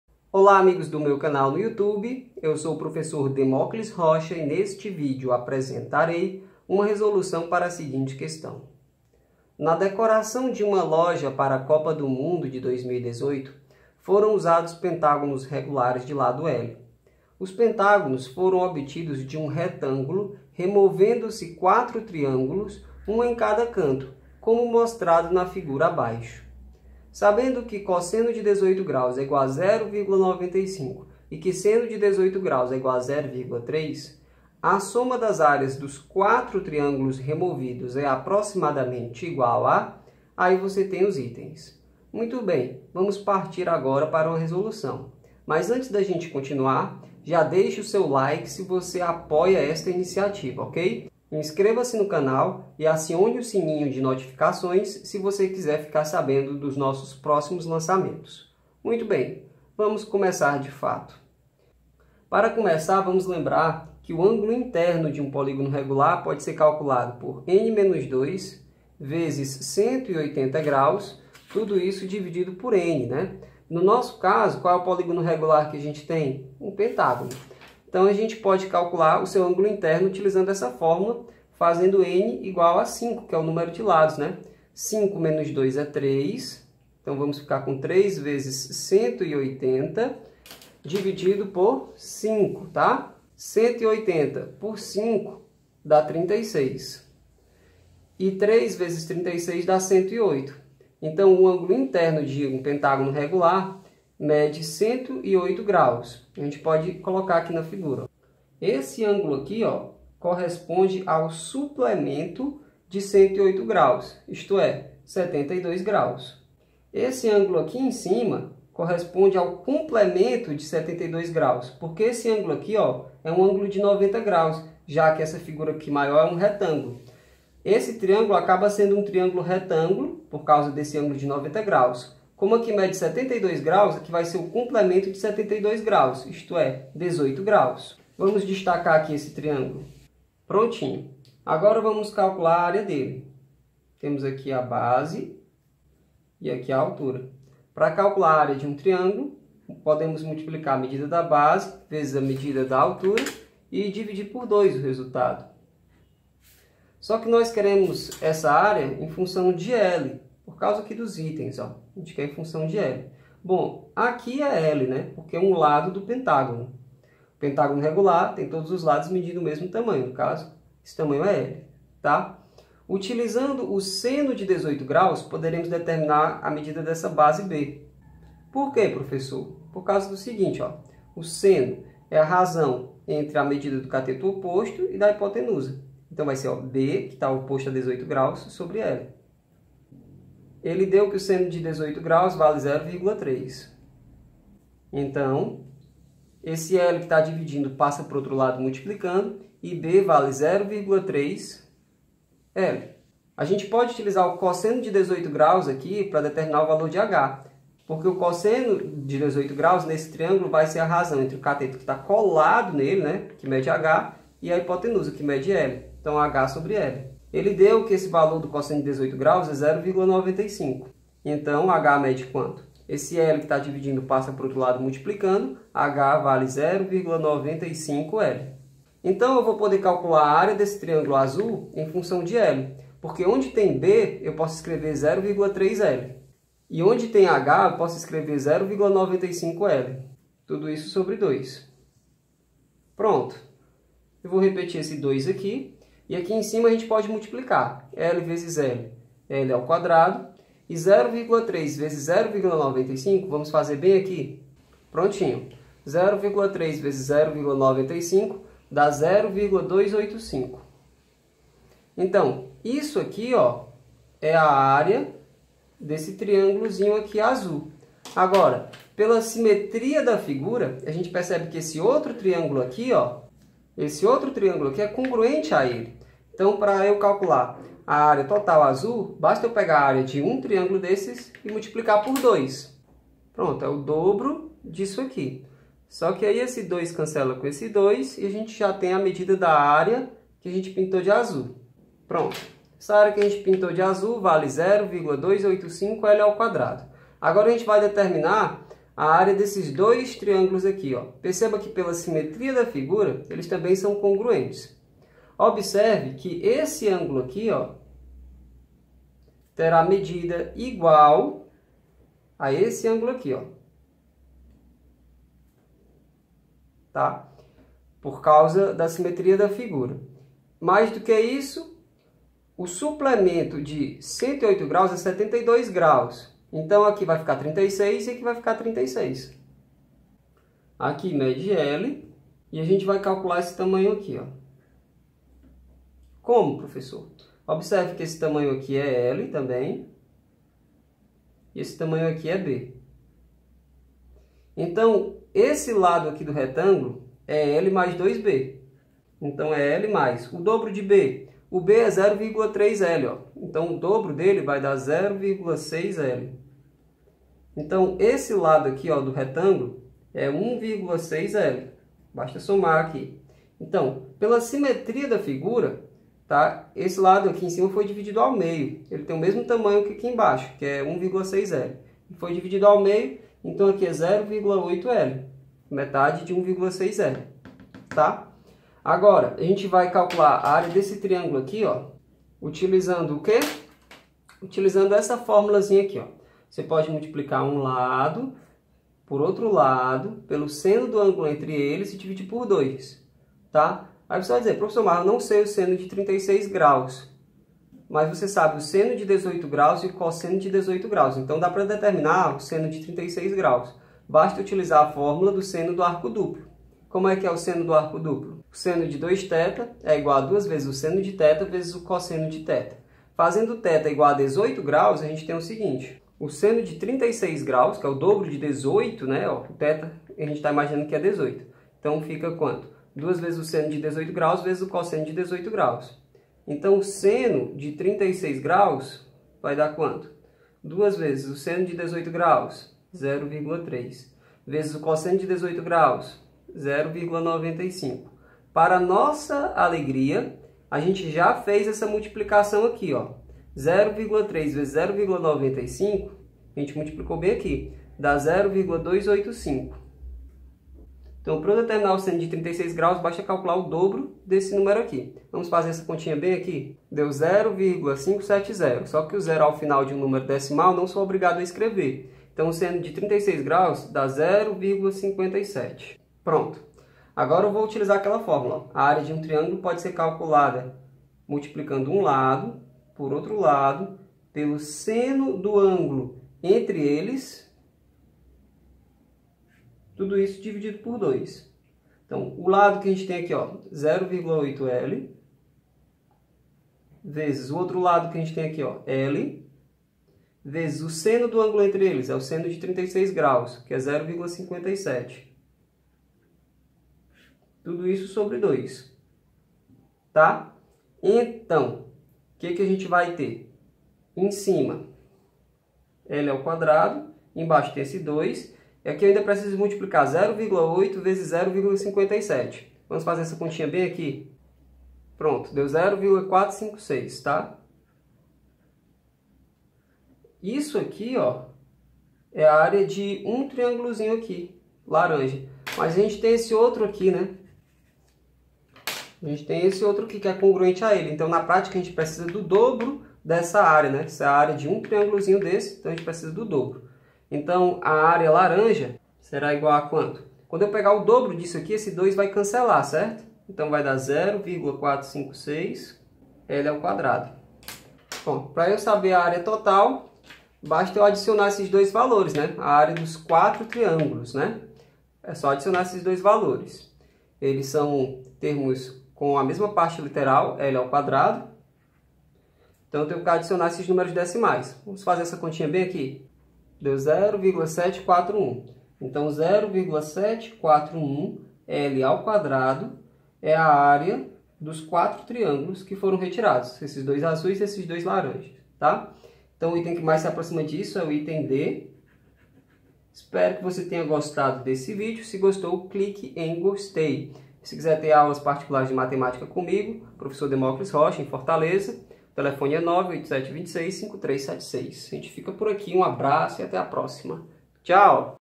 MEDICINA- UNIFOR - 2018.2 - Questão 9 - Trigonometria e Polígono Regular
Questão 9. Na decoração de uma loja para a Copa do Mundo de 2018, foram usados pentágonos regulares de lado l. Os pentágonos foram obtidos de um retângulo removendose quatro triângulos, um em cada canto, como mostrado na figura abaixo.

Sabendo que cos 18º = 0,95  e que sen 18º = 0,3, a soma das áreas dos quatro triângulos removidos é aproximadamente igual a

(A) 0,352l²
(B) 0,487l²
(D) 0,744l²
(E) 0,951l²

Sony Vegas

Atube Catcher

MICROFONES

Lapela Sony: ECMCS3